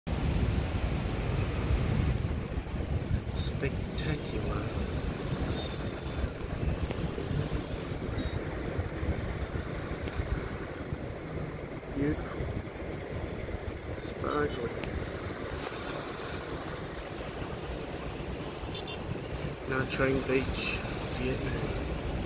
Is this one of Travel's best beaches?
Spectacular. Sparkling. Must share again, in video: Morning, Nha Trang beach, Vietnam.

(Please don't all come at once - you'll push up the prices I pay 😄)